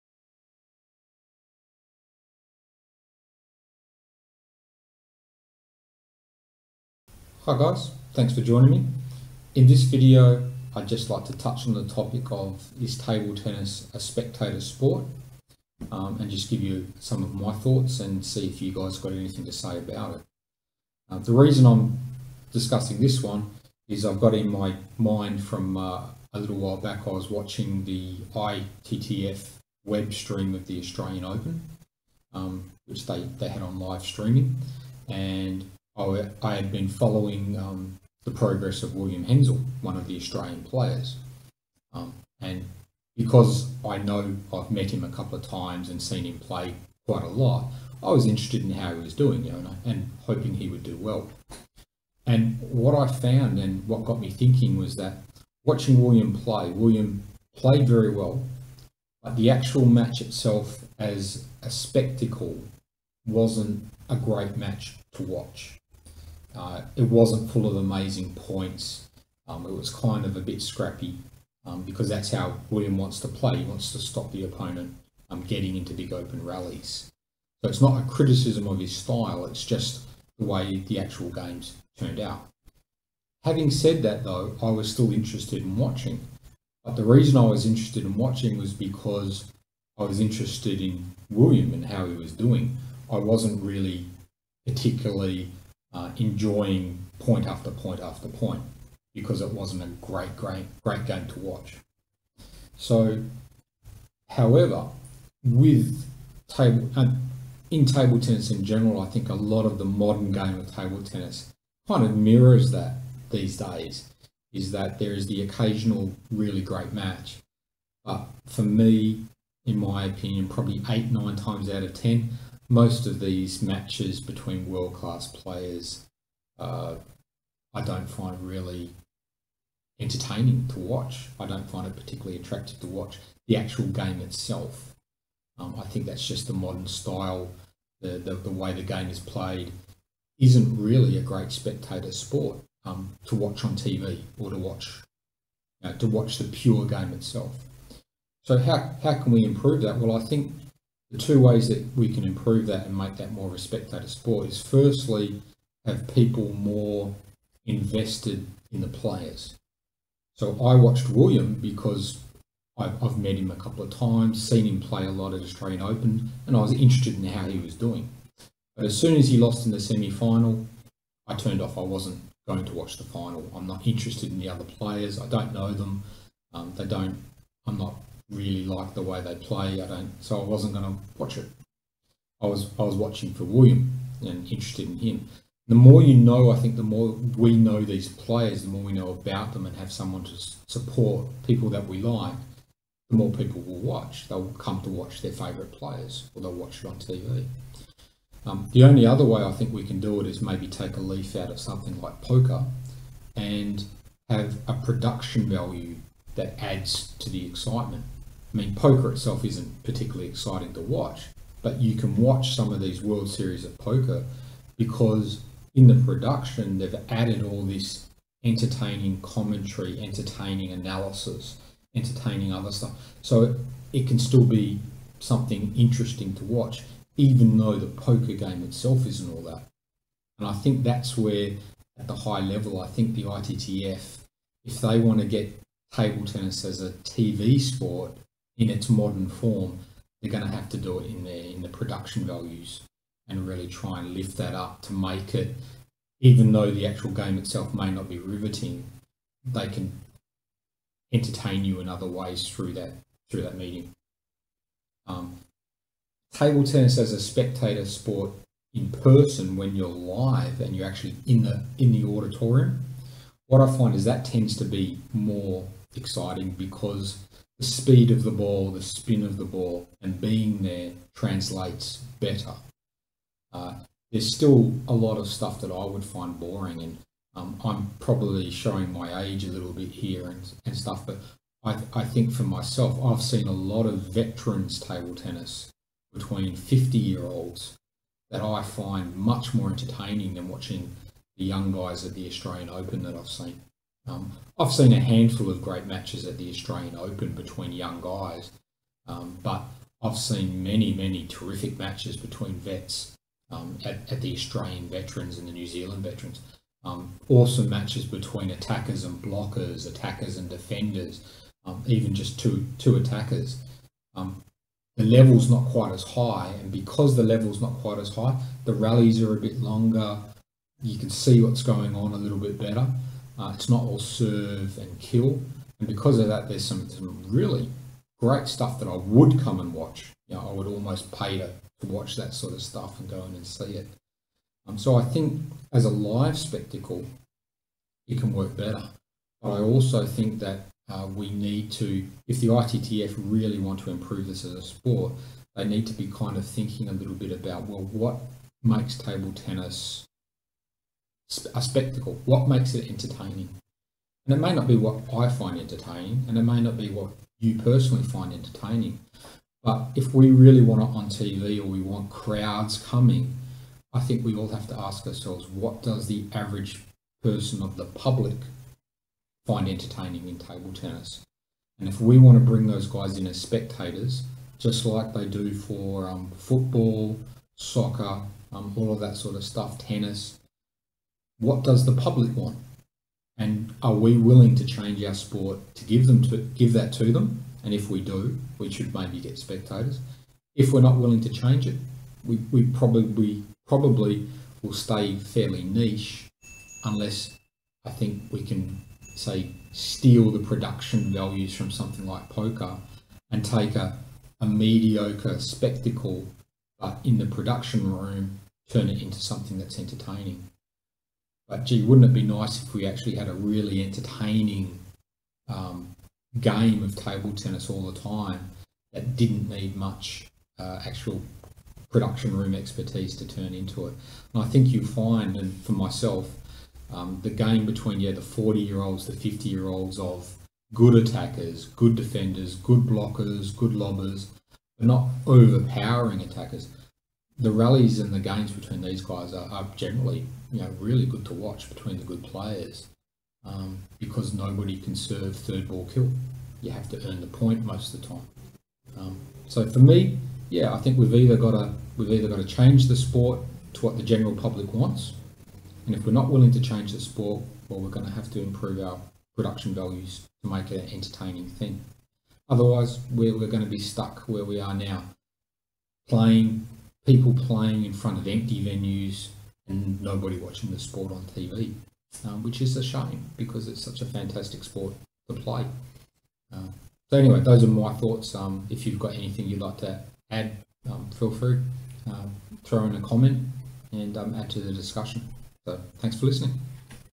Is Table Tennis a Spectator Sport?
Is table tennis a spectator sport, and if not, what can we do to get the crowds back watching ping pong?